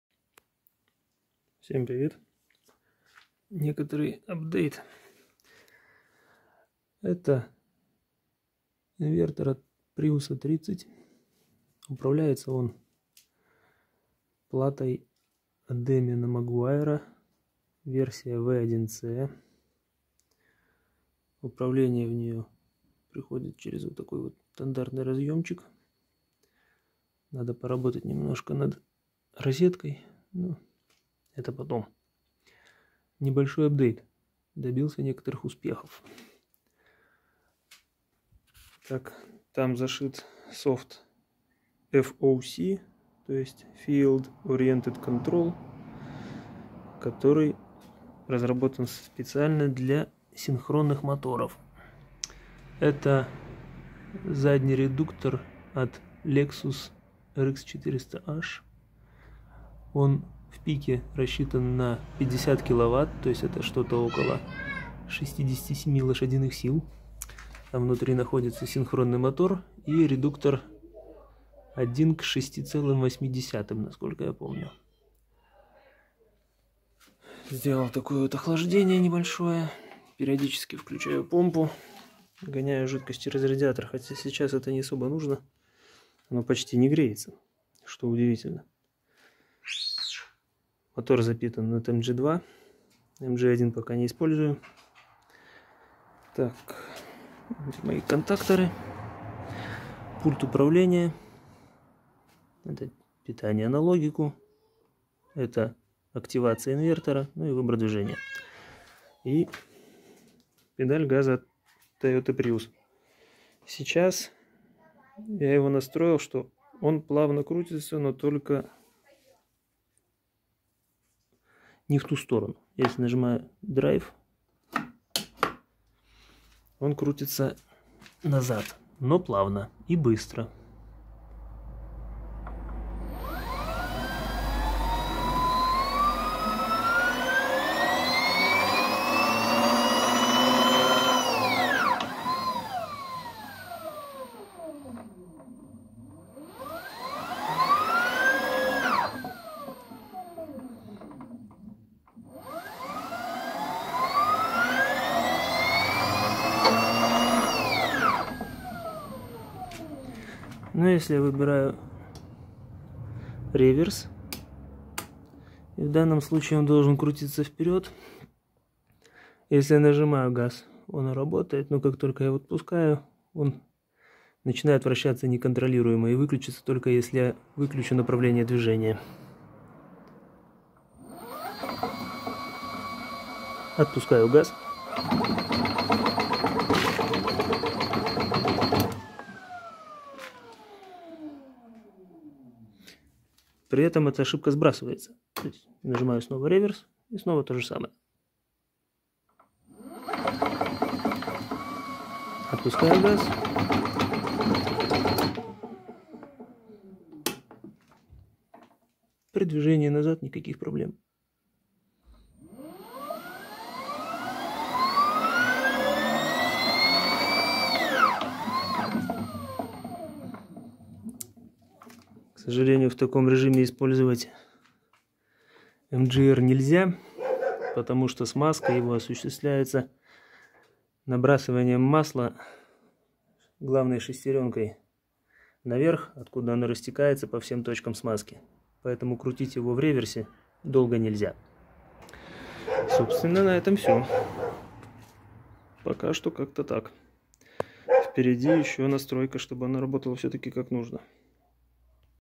FOC software. Test #3. Электромобиль своими руками.
Fine tuning in progress. FOC is pretty hard to tune correct.
Тонкая настройка в процессе. FOC - крайне чувствительная к нюансам система управления.